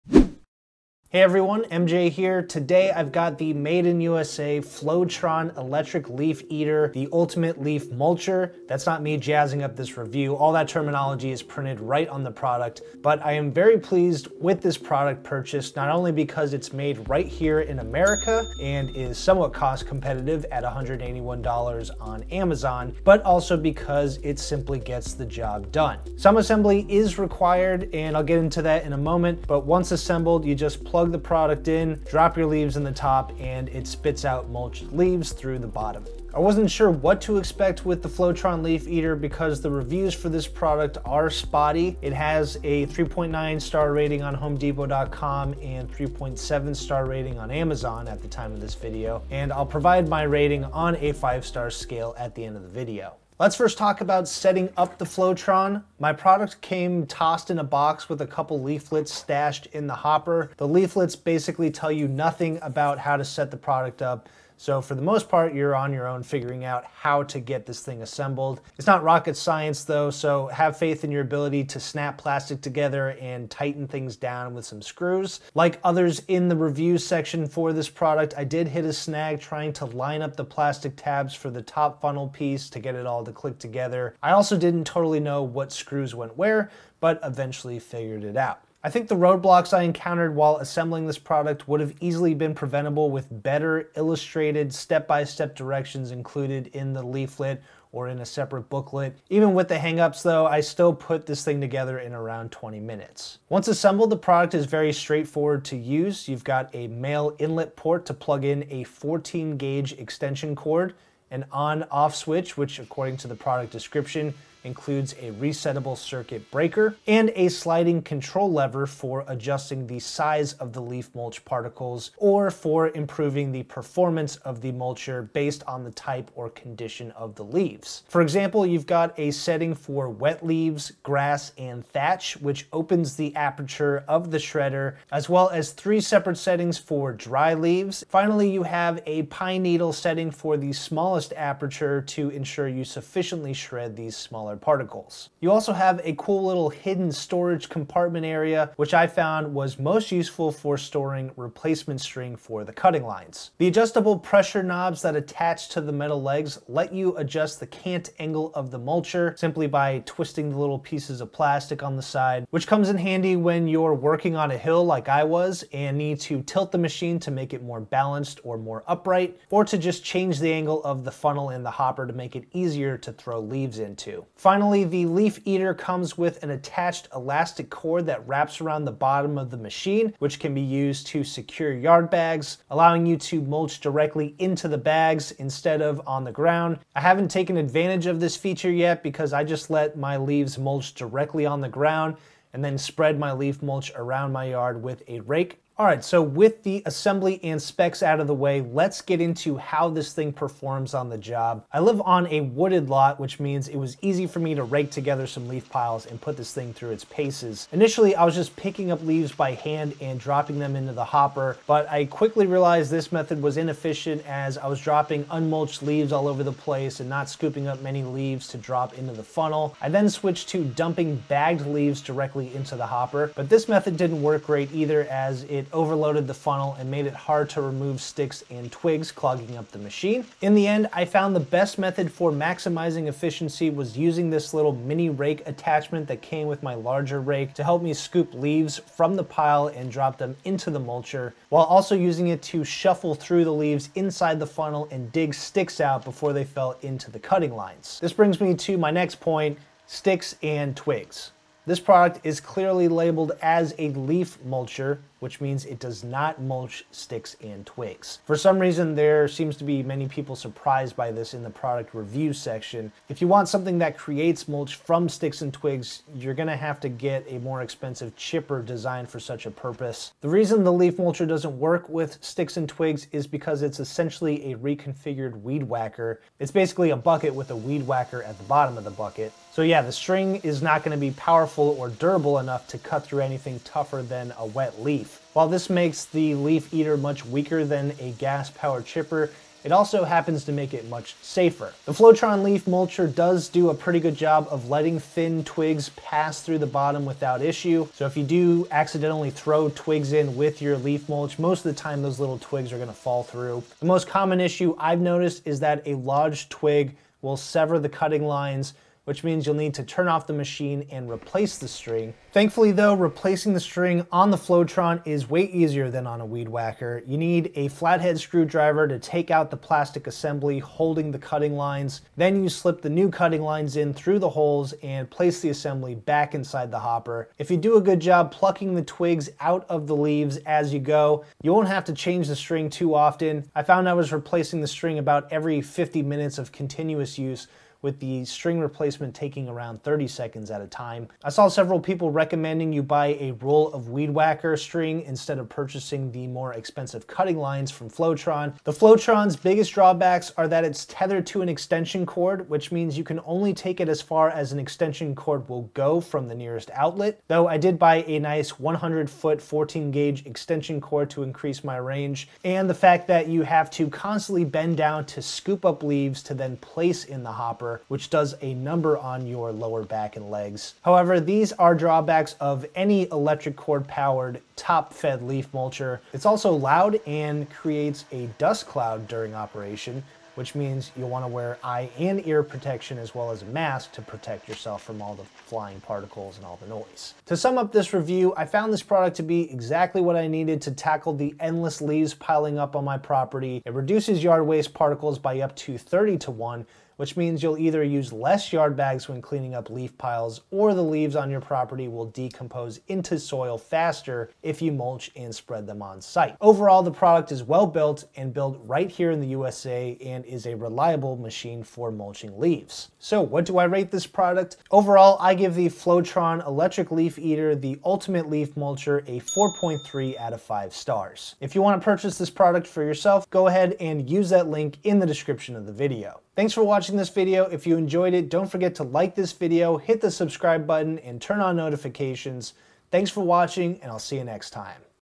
Flowtron LE-900 Electric Leaf Shredder | Made in USA Leaf Mulcher | Complete Product Review 2023
In this product review, I take an in-depth look at the Made in USA Flowtron LE-900 Electric Leaf Shredder: The Ultimate Leaf Mulcher! This is a great product for anyone looking to mulch leaves for their yard, or to simply reduce the size of their leaf piles to save more yard bags. It's all a more sustainable alternative to gas-powered leaf eater or chipper machines, especially since it doesn't use a battery and plugs directly into a wall outlet.

Mulched leaves have many benefits: they break down into soil faster than un-mulched leaves, are a great addition to composts, and can even be sold as a product to gardeners, landscapers, etc. Plus, mulching your leaves helps reduce waste in landfills.

This product is made right here in America, making it a great alternative to Made in China or other foreign made products.

0:00 - Intro to the Flowtron Leaf Mulcher
0:36 - Online Reviews
0:53 - How Easy Is It to Assemble the Flowtron Leaf Eater?
1:43 - Flowtron LE-900 Product Features Overview
3:15 - How Well Does Flowtron Mulch Leaves?
4:08 - How Well Does Flowtron Mulch Sticks and Twigs?
5:03 - Replacing the Flowtron Cutting Lines/String
5:48 - Weaknesses of Flowtron Leaf Mulcher
6:27 - Review Summary